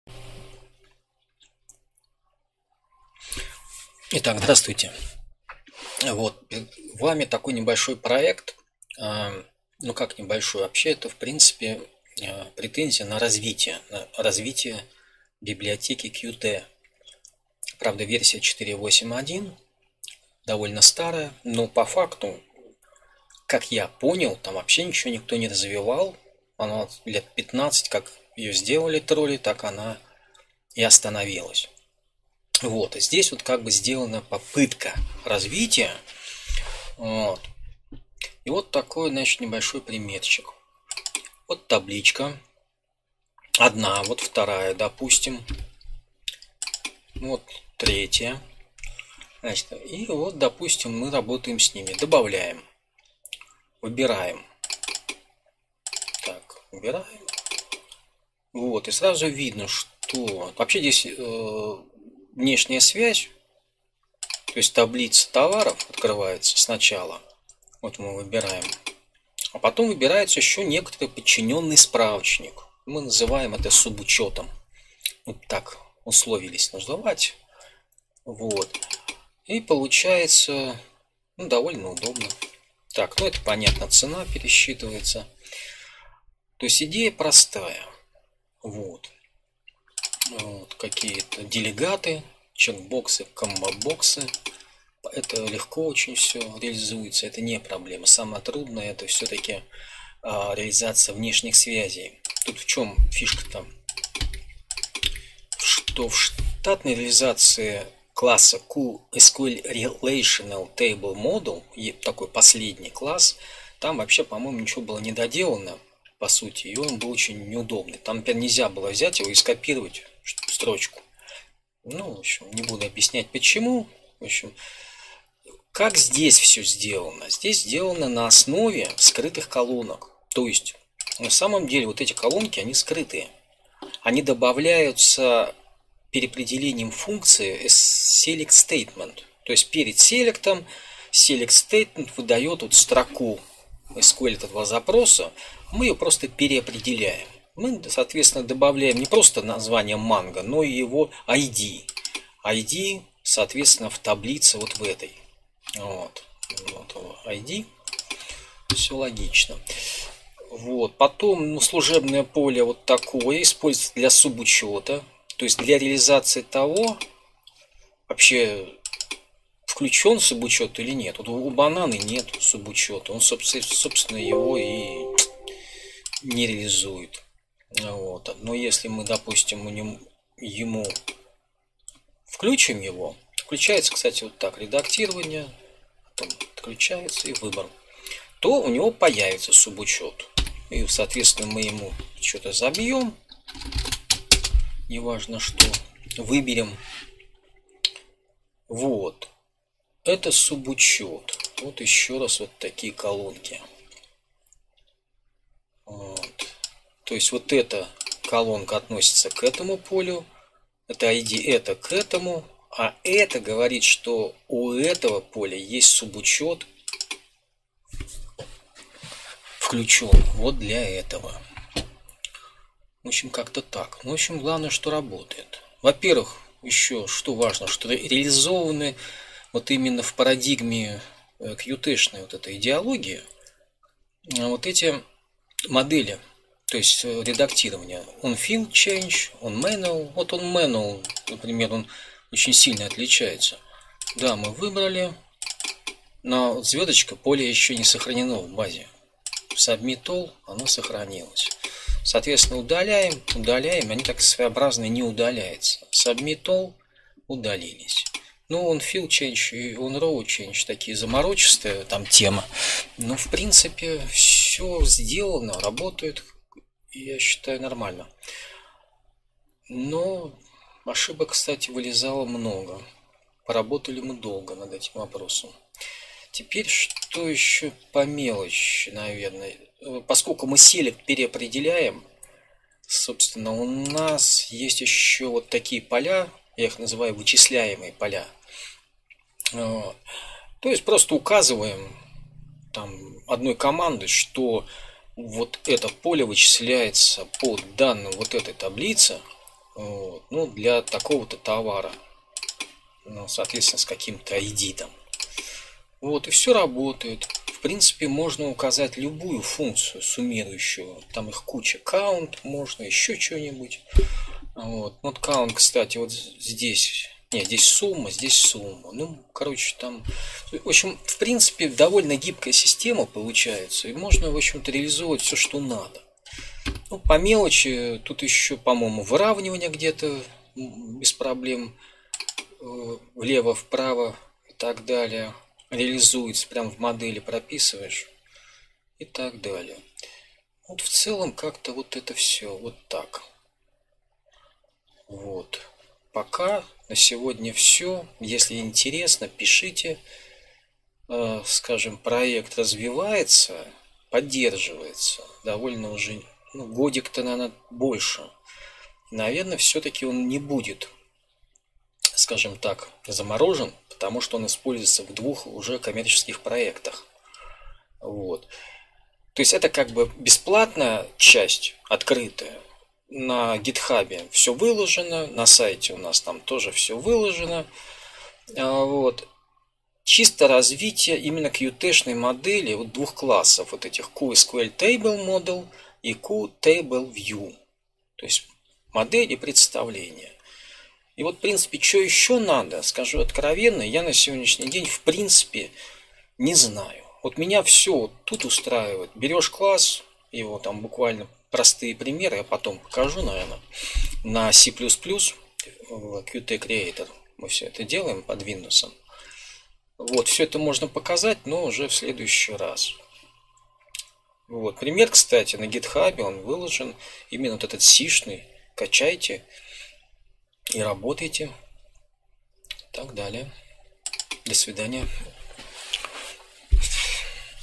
Демонстрация работы с таблицами базы данных, наследование от классов QSqlTableModel и QTableView.
Открытый свободный проект, демонстрирующий удобное управление таблицами базы данных (тестировали пока с QSLite).
Написан на С++ (98) в Qt (4.8.1). Реализовано открытое наследования классов QSqlTableModel и QTableView.

Основная идея быстро настраивать связанные таблицы (внешние связи) и далее все операции такие ка вставка строки, удаление строки, редактирования полей, работает автоматически.

Также имеем возможность настраивать субучет для колонки. То есть после выбора основного значения может автоматом открываться диалог выбора подчинённого справочника, например размер товара одежды.

Проект традиционно выложен на гитхабе,  на Хабре есть статья, и на нашем сайте kkmspb.ru тоже .